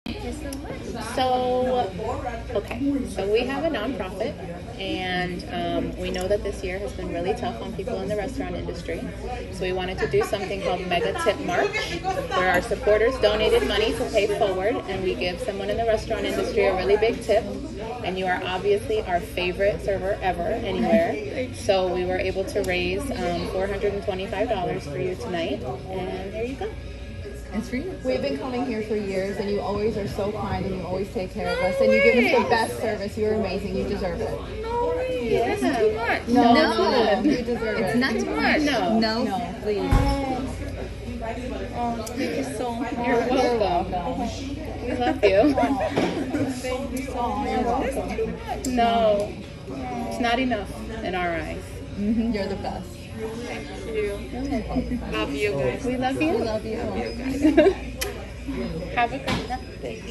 100 People Project Surprise Waitress with Large Tip
Full story on Coral Springs Talk.